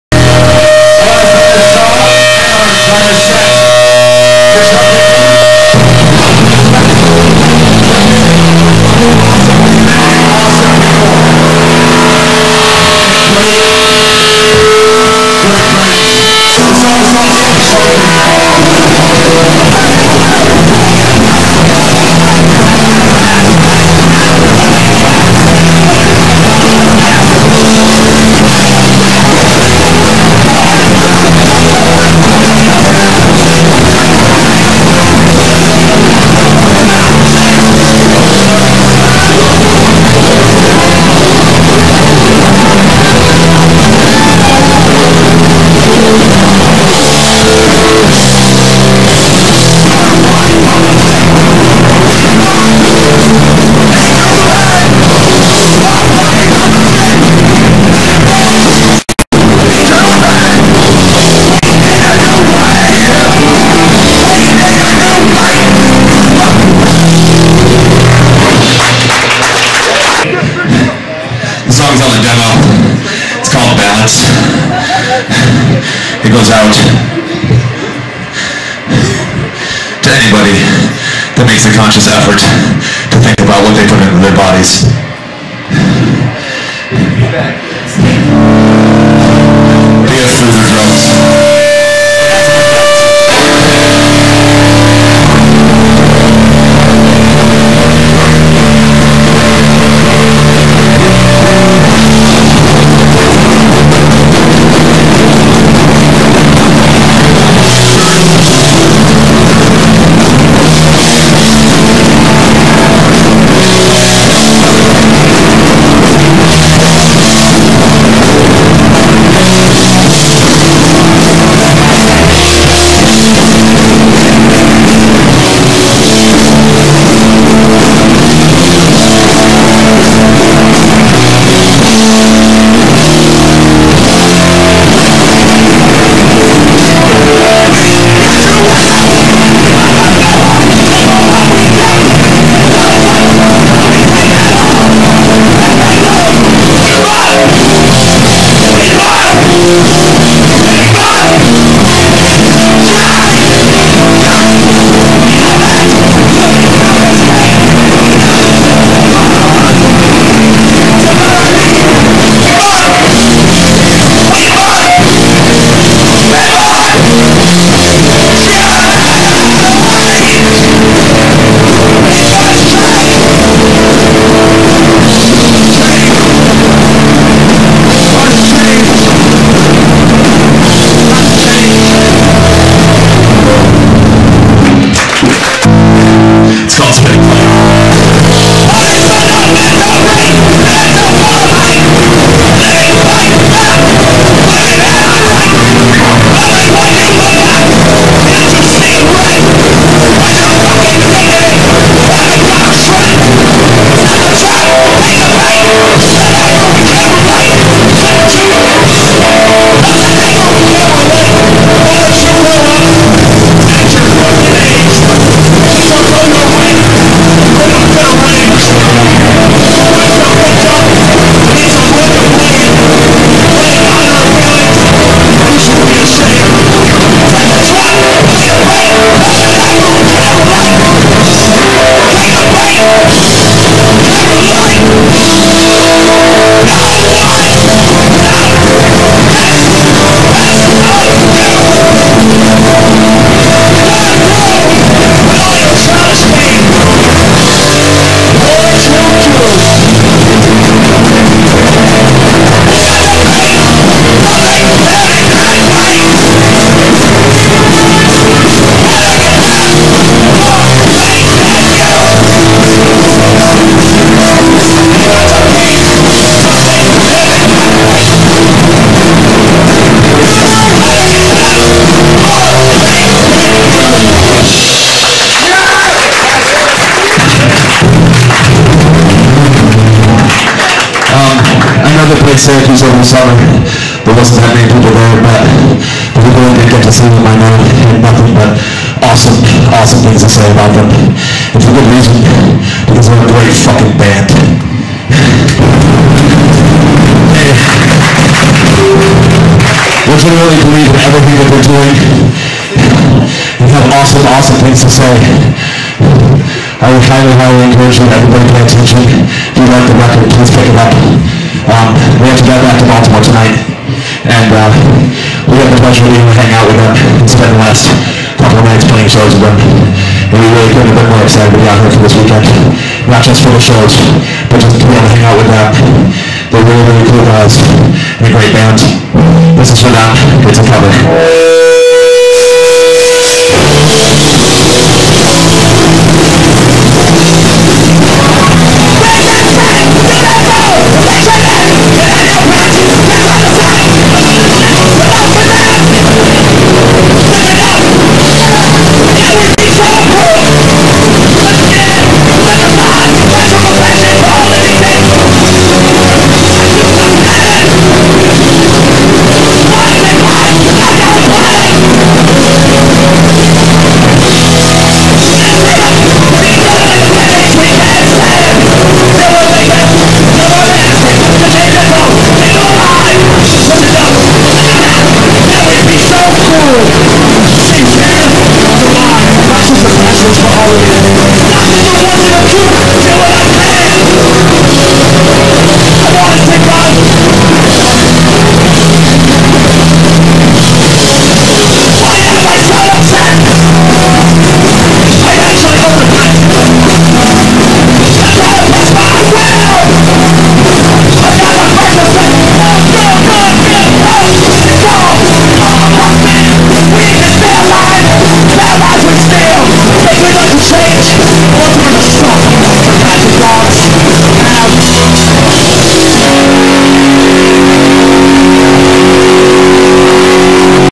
Not Sorry 11-22-09 Syracuse, NY pt. 1
Seattle Hardcore Band "Not Sorry" plays the Wescott Community Center in Syracuse, NY (sorry about the poor audio)